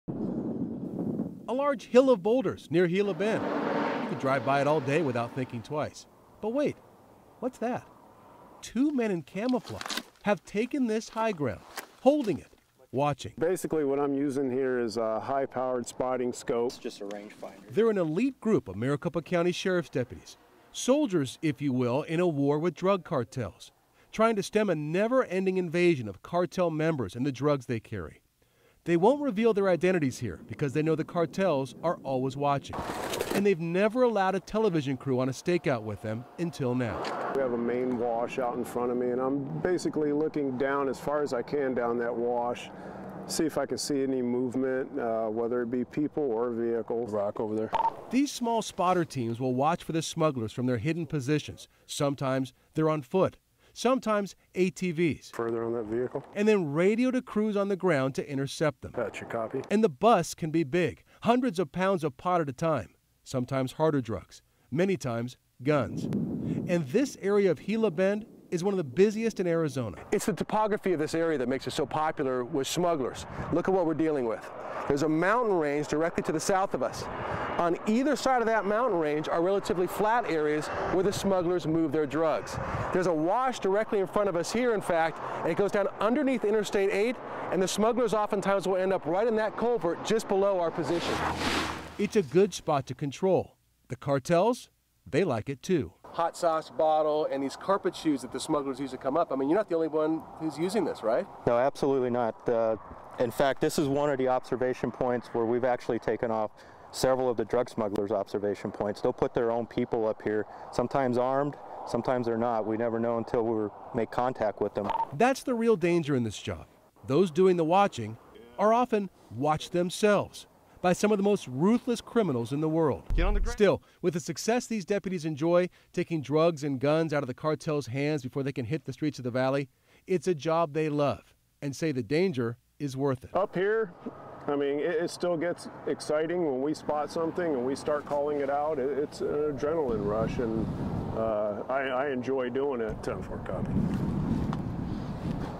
Desert Drugs
Troy H and I hang out with the sheriff dept as they hunt for drugs.